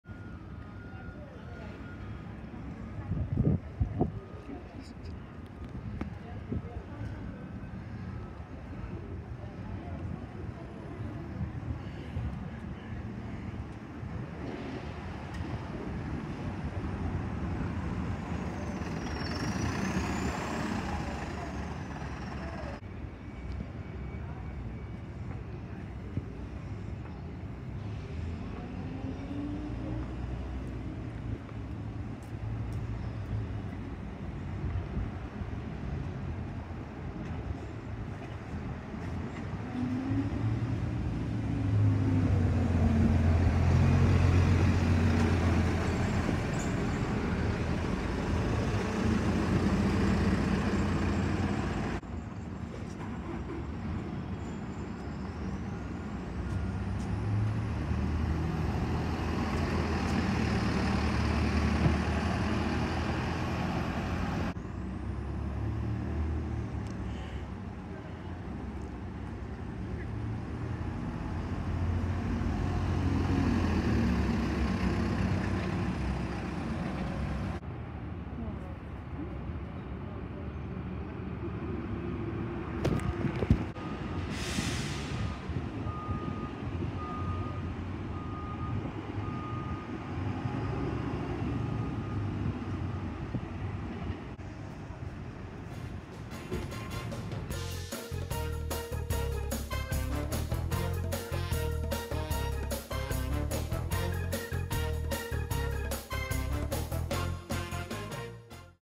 Buses at Keighley Bus Terminal 20th October 2020
The following buses are in this video:

Optare Solos
Volvo B7TL Gemini 2s [BAD FUMES]
Volvo B7RLE Eclipse 1 & 2 Urbans
Mercedes-Benz Sprinters
Optare Versas
Plaxton Presidents